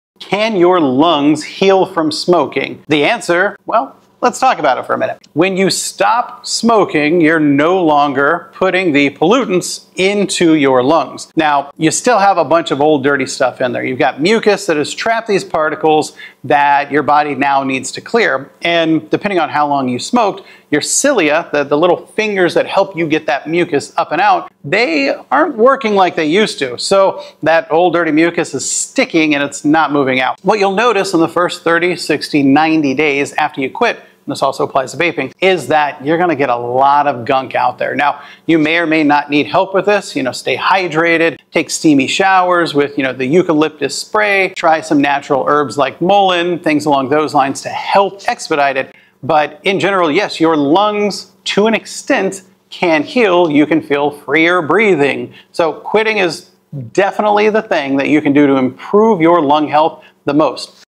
Do Your Lungs Heal After Smoking?
One of the most common questions I get is: can your lungs actually heal after smoking or vaping?
The answer is yes — to a point. And understanding how that healing happens makes the process a lot less scary 😮‍💨.

When you quit smoking or vaping, the first big win is simple: you stop adding new pollutants. But that doesn’t mean your lungs are instantly clean. For most people, there’s still old, dirty mucus sitting in the lungs — mucus that trapped smoke particles, chemicals, and debris over time.

Here’s where things get uncomfortable.

Smoking damages the cilia — the tiny hair-like fingers in your lungs that normally move mucus out. If you’ve smoked for a long time, those cilia may be sluggish or barely working. So when they start waking back up after you quit, all that old mucus finally starts moving.

That’s why in the first 30, 60, or even 90 days, many people notice coughing up a lot of “gunk.” It feels gross — but it’s actually a sign that your lungs are clearing themselves.

To help that process along, here’s what I recommend:

💧 Stay well hydrated
Water helps thin mucus so it’s easier to move out.

🚿 Take steamy showers
Steam helps loosen mucus. Adding something like eucalyptus can make breathing feel more open 🌿.

🌱 Consider natural support
Herbs like mullein can help support mucus clearance and make the process more comfortable.

The most important takeaway is this: quitting is the single best thing you can do for your lungs. Over time, many people notice freer breathing, less tightness, and better overall lung function.

Healing isn’t instant — but it is real.